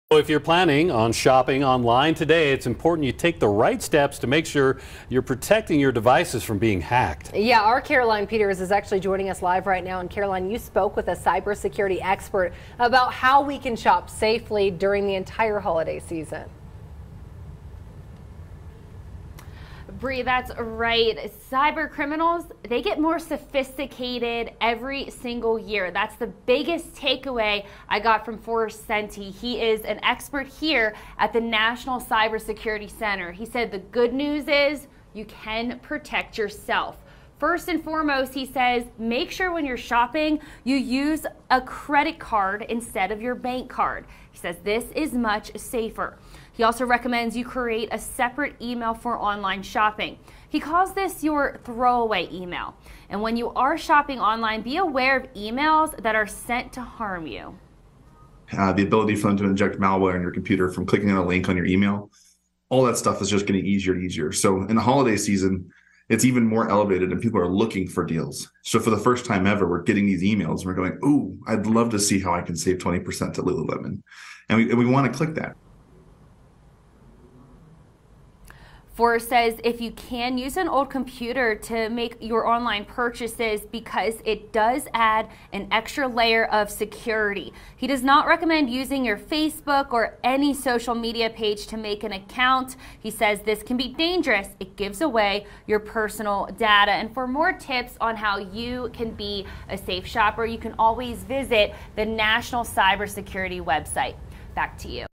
Shop safe and local for Cyber Monday
Cyber Monday is upon us and while many people look to get the best deals online, small businesses can sometimes be overlooked. That is why some local businesses are working on building their online presence.